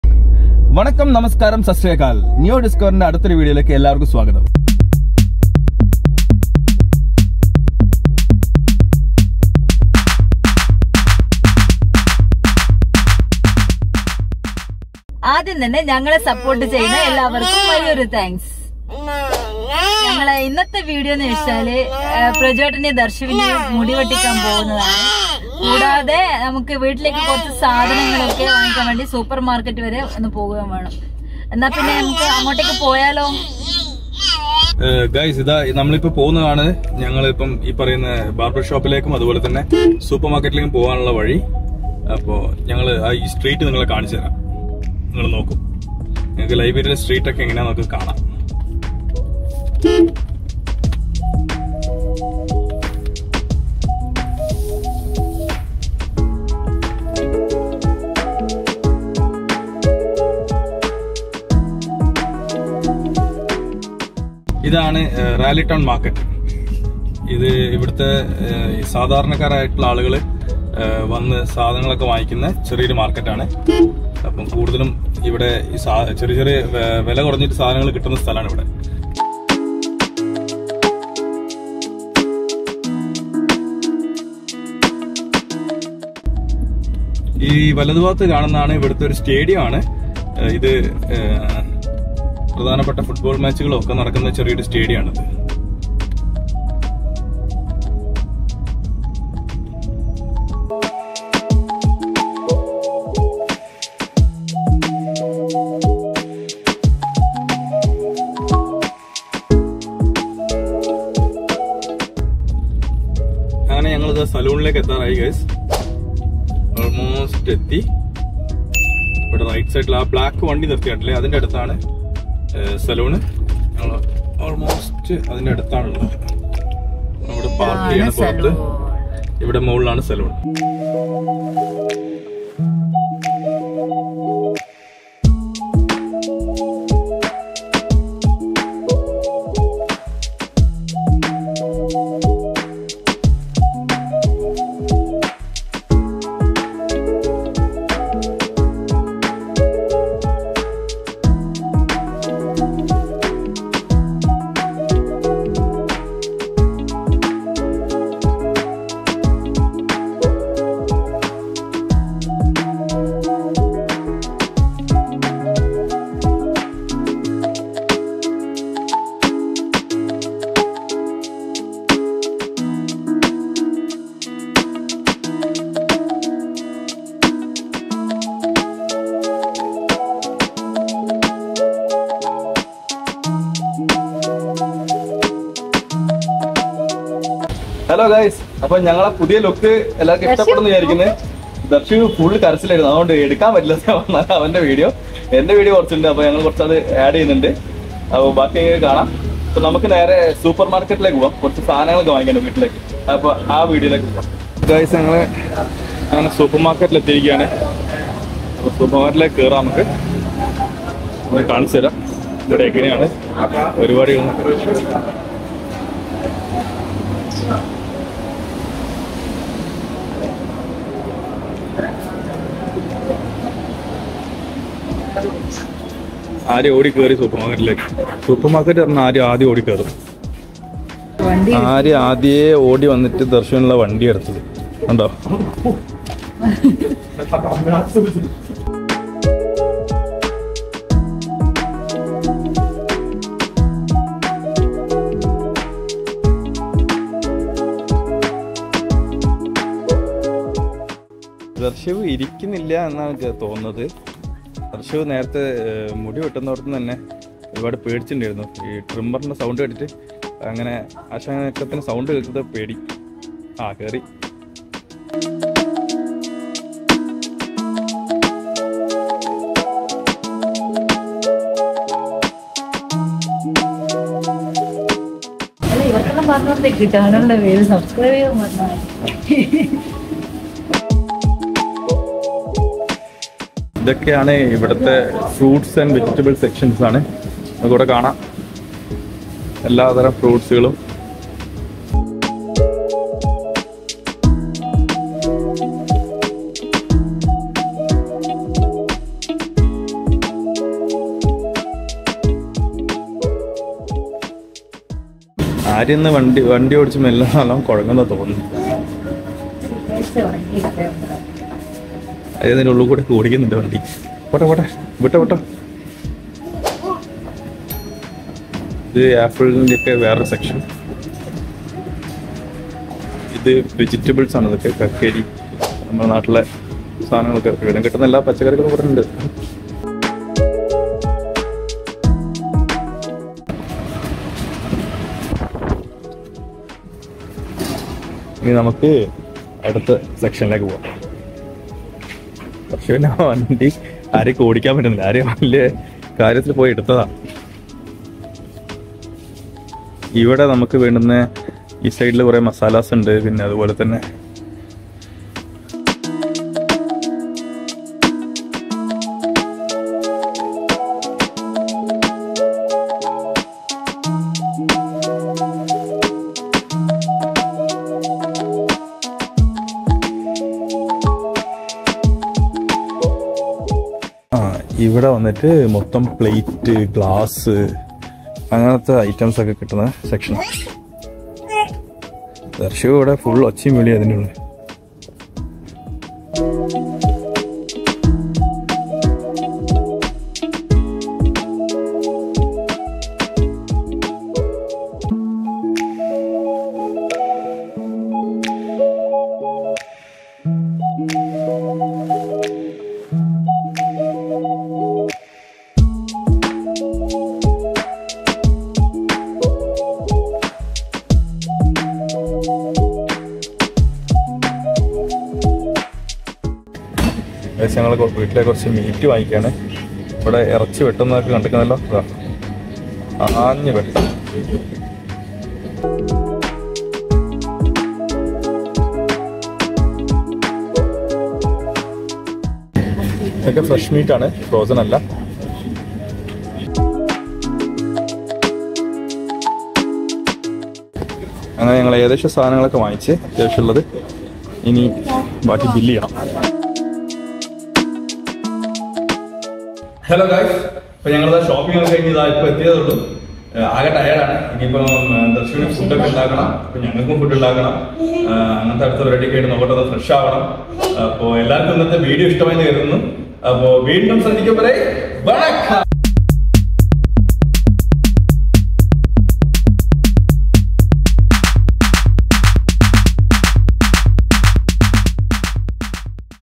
Exploring Liberian Life | ലൈബീരിയൻ ജീവിതം | Haircut and Supermarket Shopping in Liberia
Dharshiv's Haircut Adventure and Supermarket Shopping in Liberia